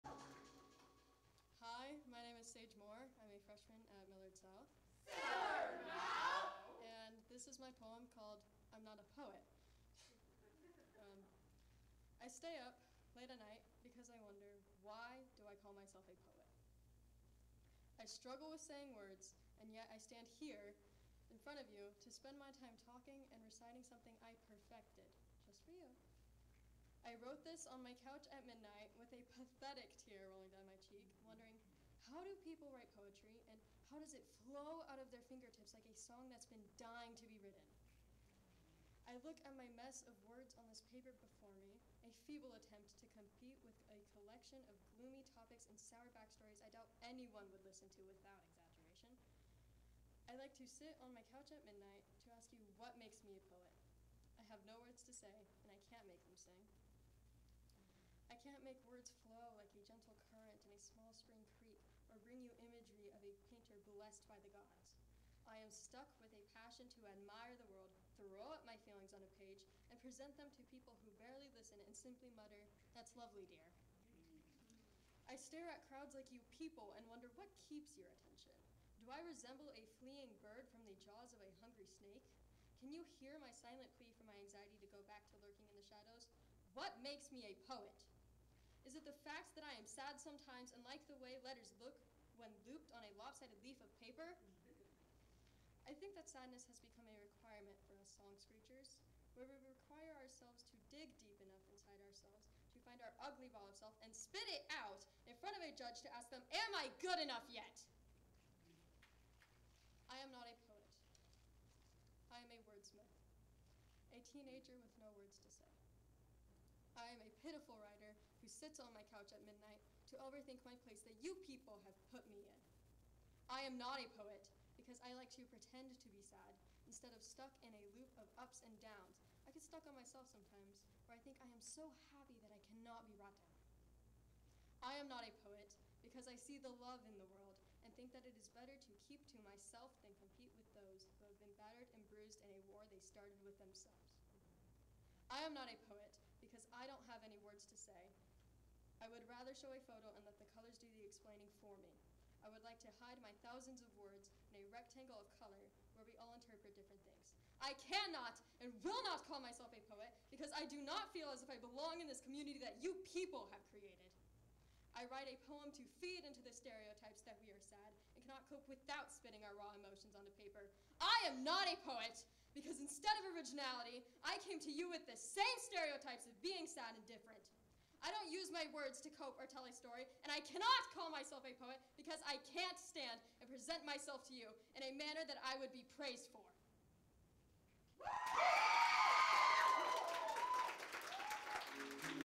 Louder Than a Bomb Great Plains 2022, Bout 2
Bout 2, March 21, 7:30pm at UNO features:
Millard South and Omaha Burke.

The heart of what LTAB is = the weekly work by our amazing teaching artists!